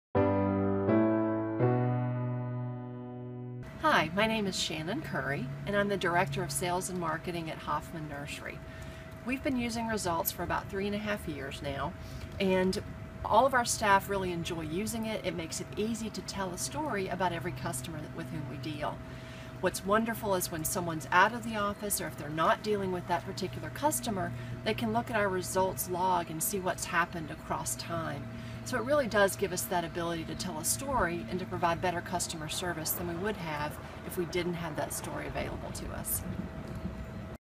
Results CRM Testimonial from Director of Sales & Marketing
The Director of Marketing at Hoffman Nurseries describes how Results CRM has impacted their business.